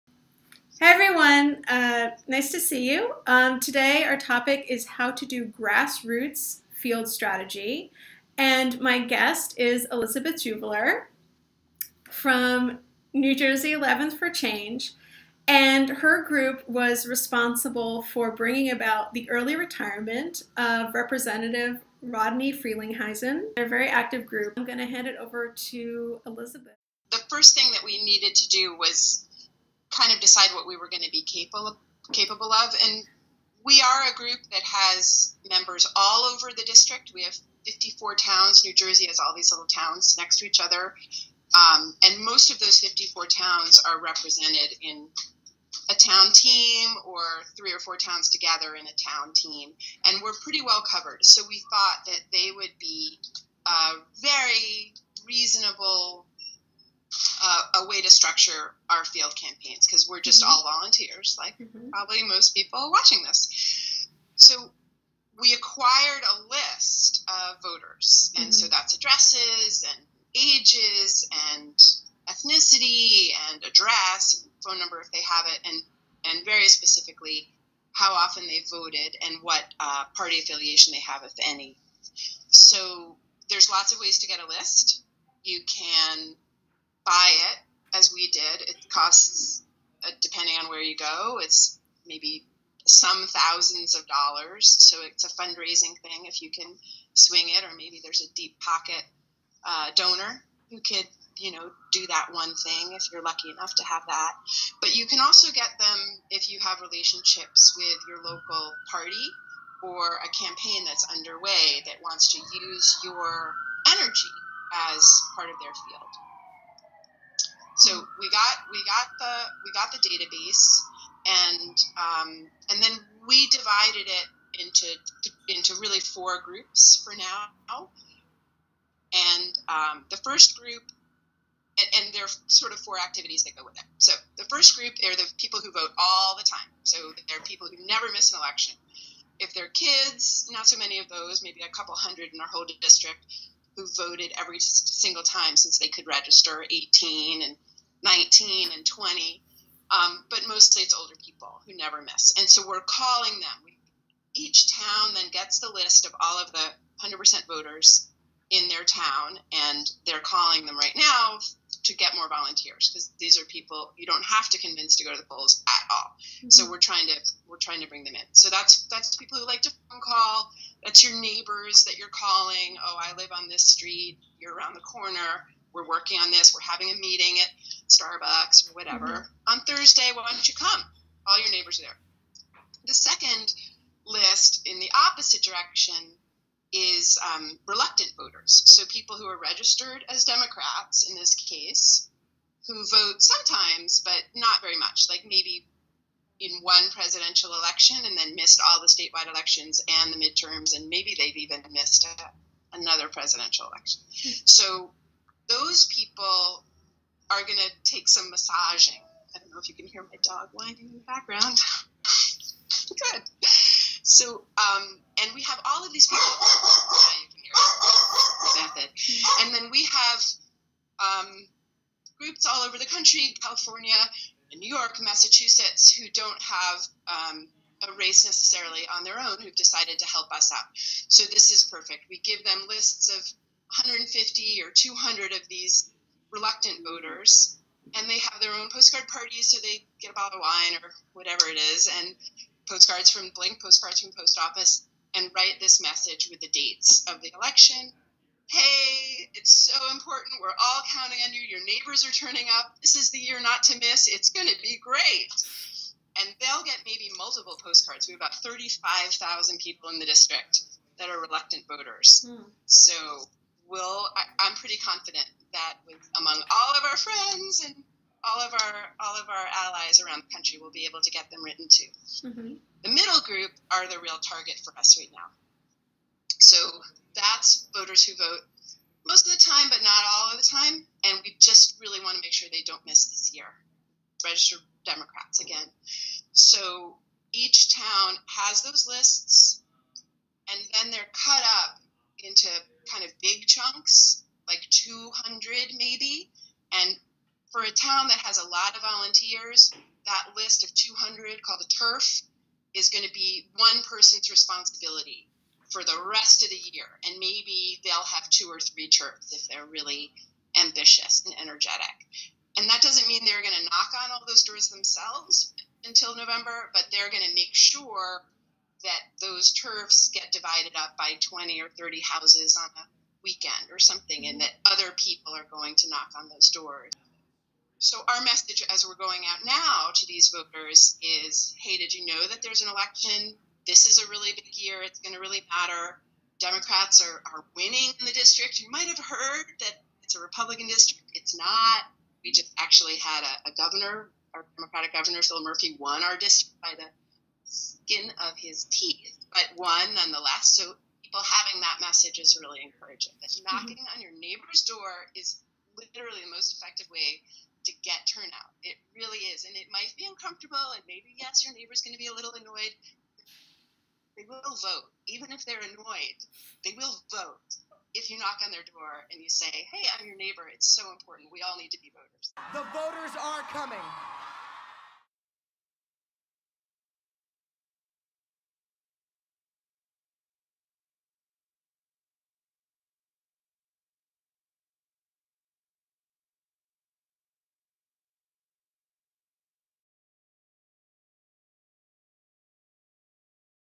Unstoppables #7: Grassroots Field Strategy
How can your group mobilize its neighbors to vote for change in the midterms?  In this information-rich video, Elizabeth Juviler of NJ 11th for Change (nj11thforchange.org)  explains how to organize your own voter outreach effort using three key tactics: phone calls, postcards, and door-to-door canvassing.